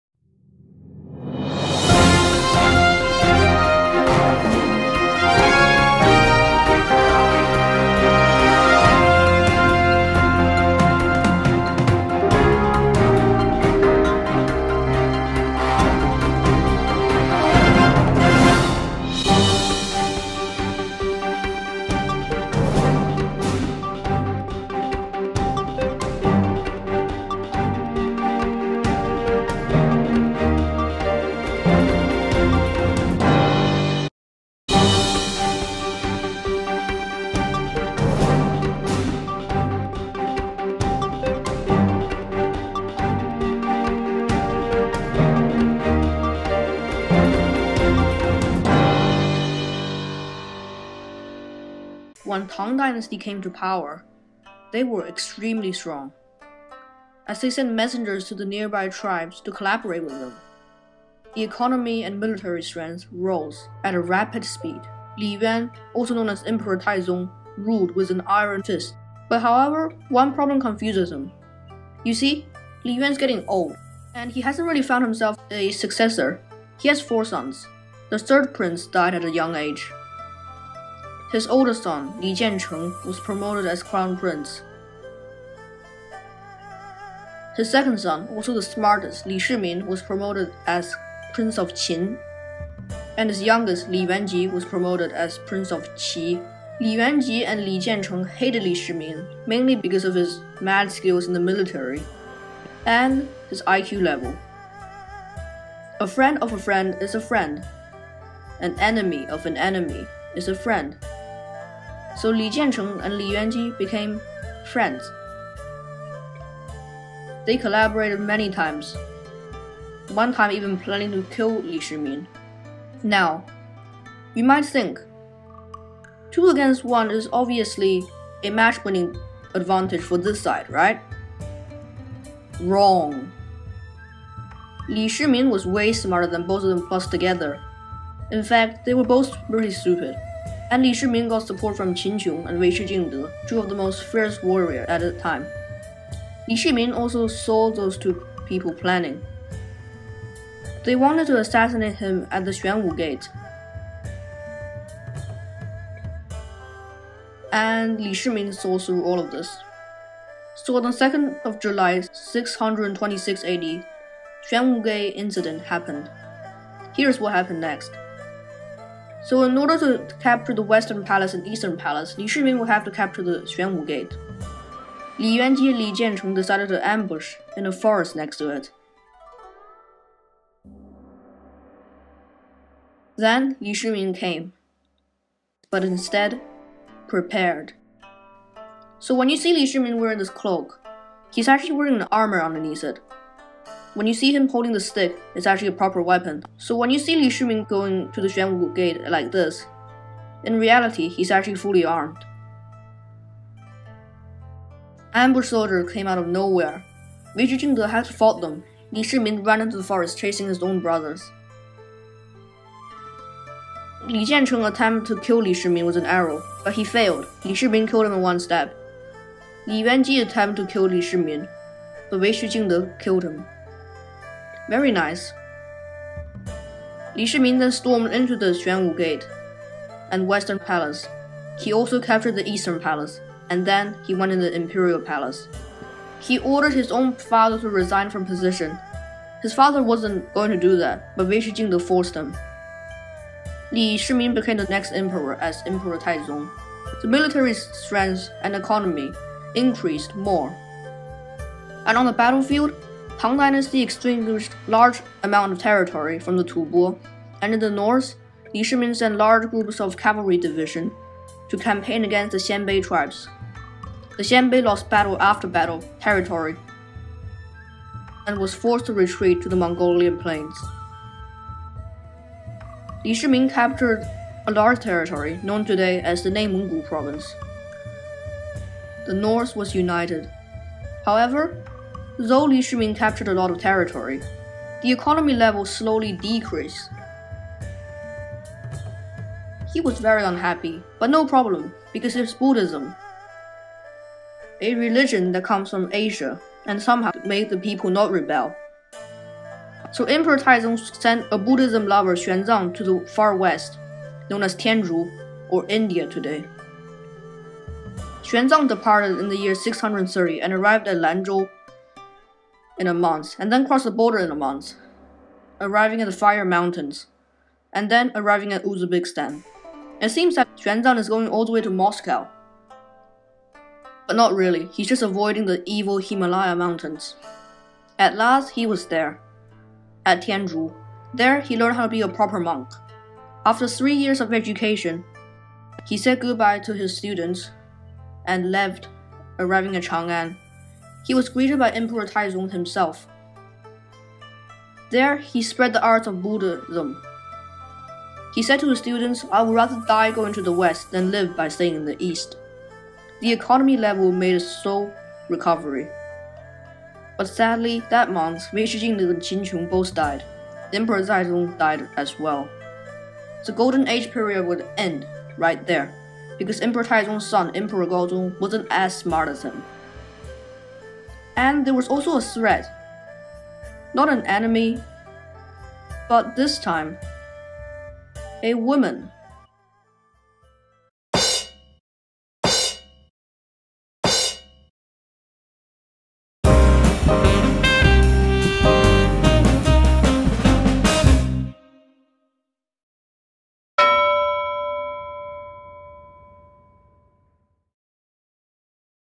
The golden ages of Tang dynasty | #4 | Mighty History
This video is about the golden age period for Tang dynasty. Under the rule of Emperor Taizu and Emperor Taizong, the country grows more powerful day by day. Like the video ONLY if you liked it.